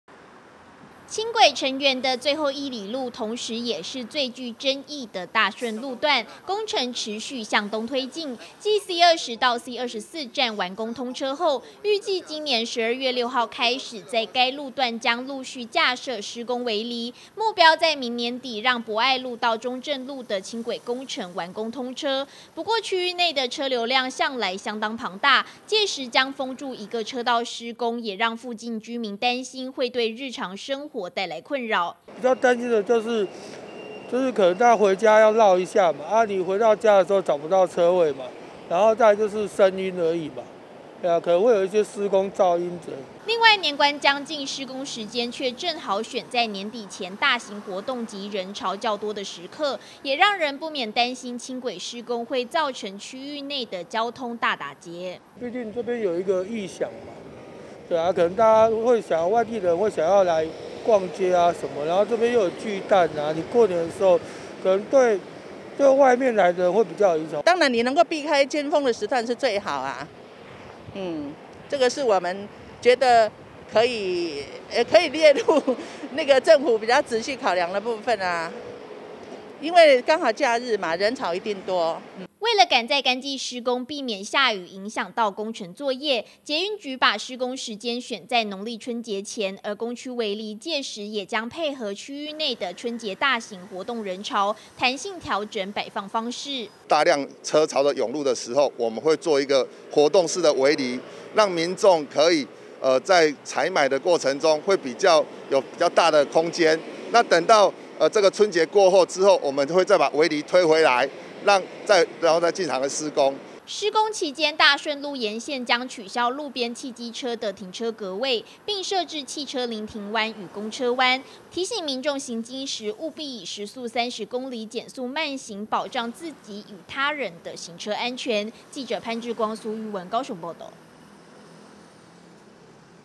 輕軌緊緊緊| 拼輕軌成圓大順路年前施工 民眾憂交通打結
高雄輕軌工程持續向東推進，預計今年12月6號起將在大順路段開始陸續架設施工圍籬，力拼明年底完工通車，達成輕軌成圓的目標。不過時間點選在農曆春節前夕，也讓民眾擔心，跨年等大型活動期間的交通問題。